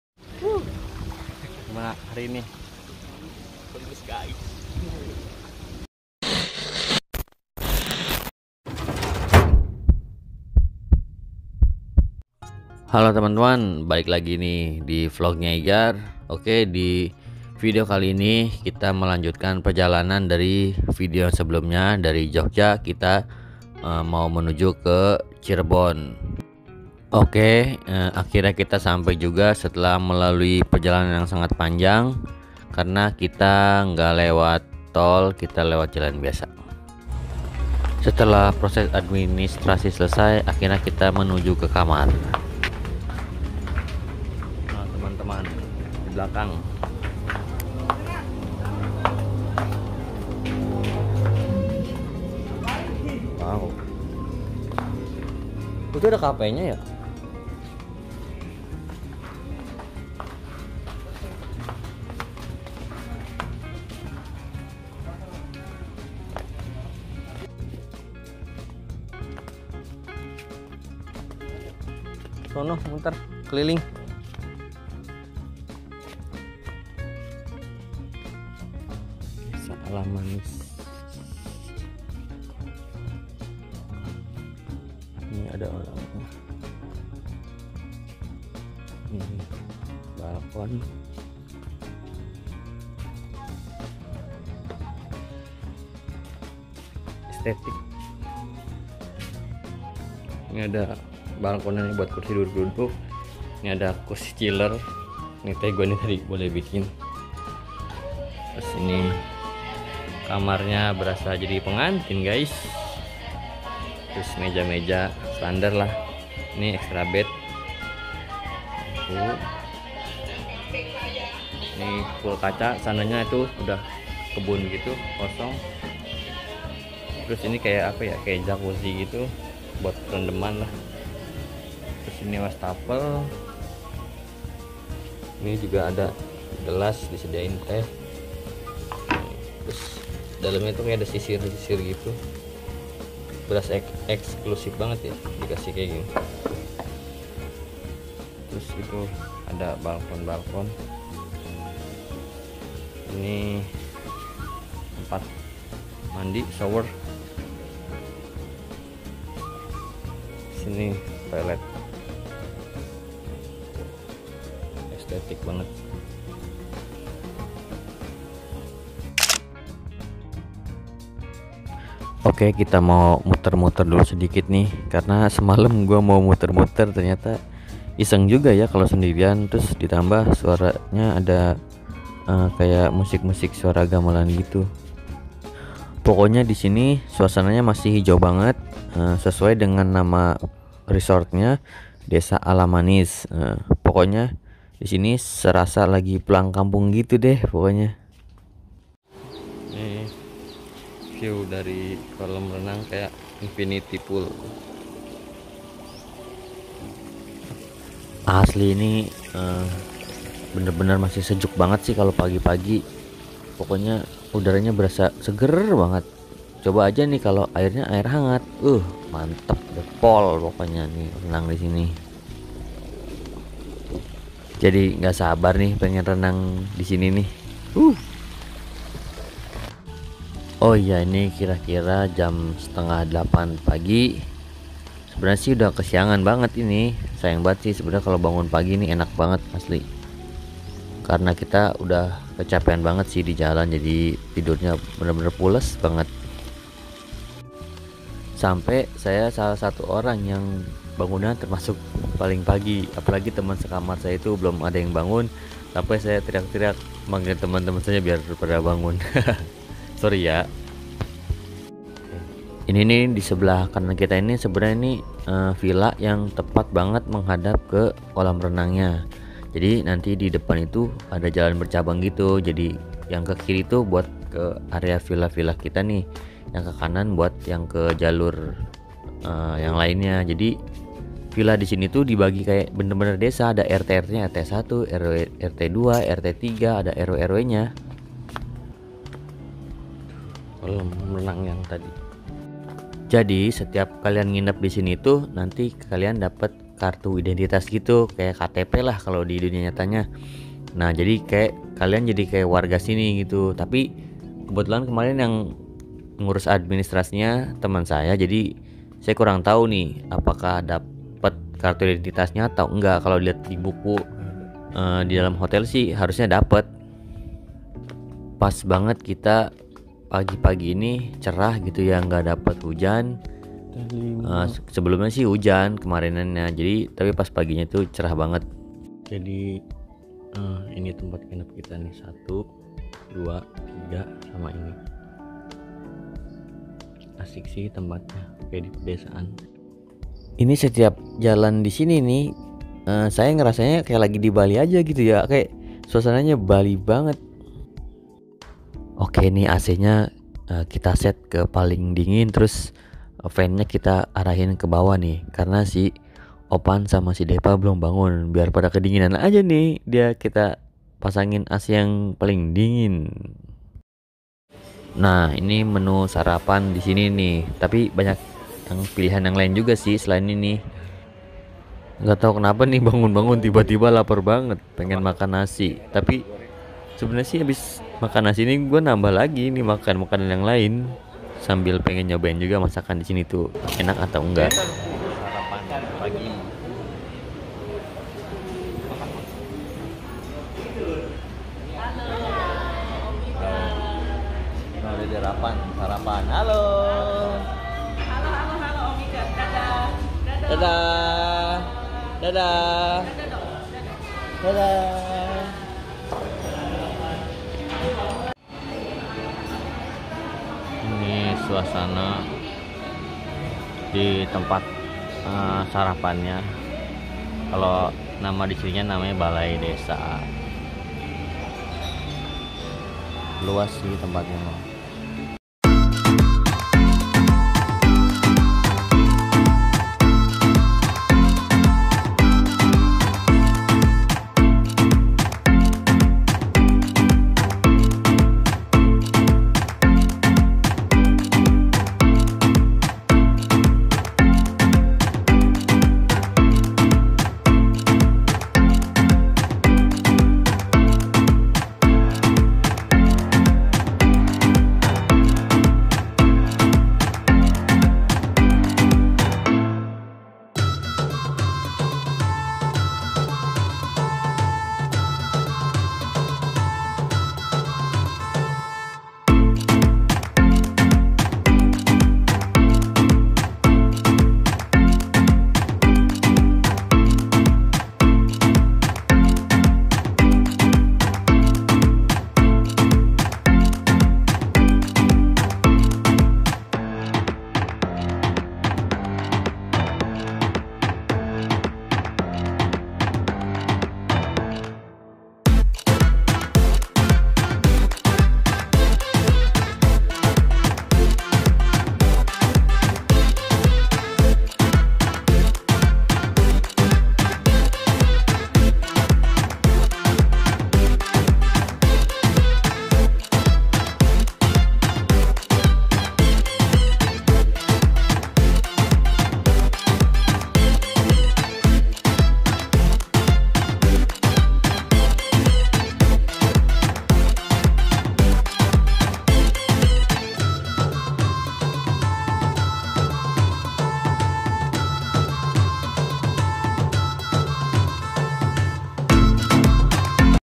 DESA ALAMANIS RESORT SEPERTI DI RUMAH NENEK
setelah sebelumnya kita menginap di jogja, akhirnya kita memutuskan untuk semalam lagi di daerah cirebon nih. letaknya ga jauh dari pintu tol cirebon